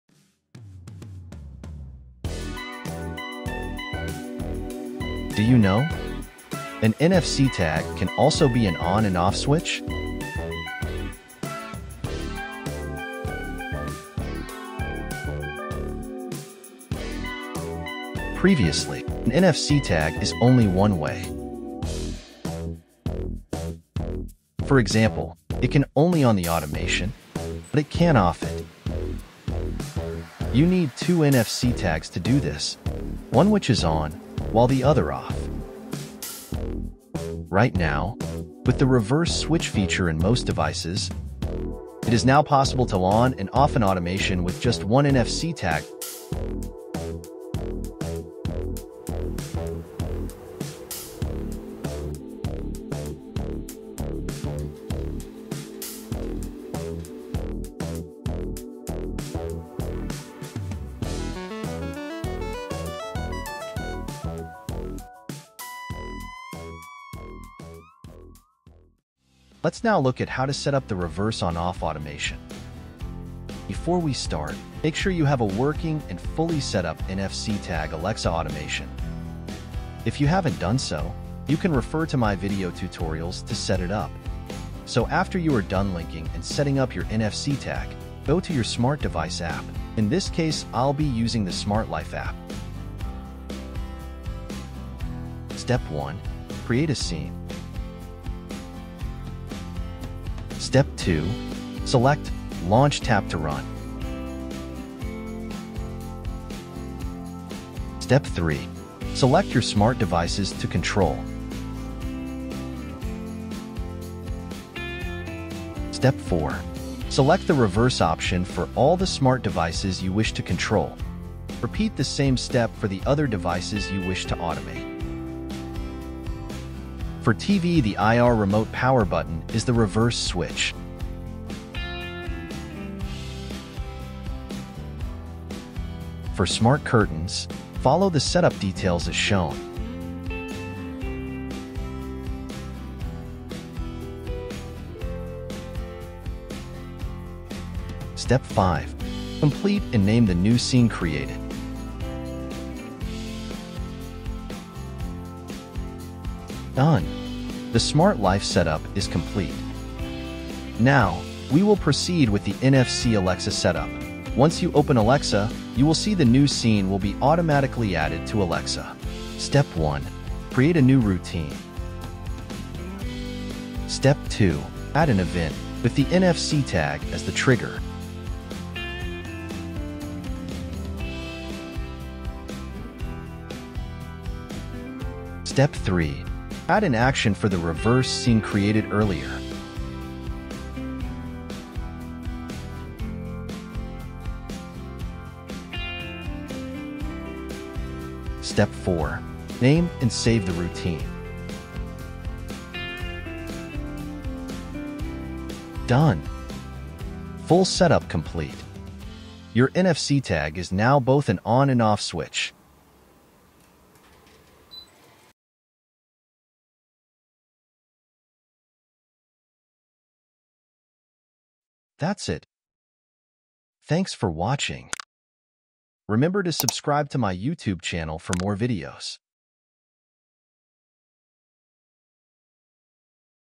How to Setup NFC Tag as an ON & OFF Reverse Smart Switch for Home Automations | Smart life & Alexa
With the Reverse switch feature in most of the Smart Devices today like the Zigbee smart switches, you can now set up your NFC Tag to both on or off the automation with the reverse feature for NFC Automations. Normally, an NFC Tag works one way, ex. it can only on an automation, but now with the reverse feature it can do both.

Make sure you have setup the NFC tag and linked to Alexa before setting up the Reverse automation. If you have not done so, this is the Video

In the Video tutorial, you need the smart device app (Example: Smart life Tuya App) and the Alexa app to setup the NFC reverse automation. Currently Alexa does not have the reverse feature in their automation, thus the automation cannot be performed directly in Alexa itself.

NFC Tag Purchase Links:
NFC Tag NTag213 (144bytes) works fine for most home automations but If you need more memory for your NFC tag, you can go for the the N215 (504bytes) or N216 (888bytes).

Link for the NFC Tags NTag 213(144 Bytes) / 215 (504 Bytes) / 216 (888 Bytes)
Appx US$0.20/pc

Link for the NFC Tags NTag 215 (504 Bytes)
Appx US$0.80/pc

0:00 NFC Reverse On Off Example
1:09 Tutorial on how to Setup the Reverse Automation
1:32 Smart Life Tuya Zigbee Setup
2:08 Reverse Switch for IR Remotes like TV Air Con
2:16 Reverse Switch for Smart Curtains & Blinds
2:42 Alexa Routine Setup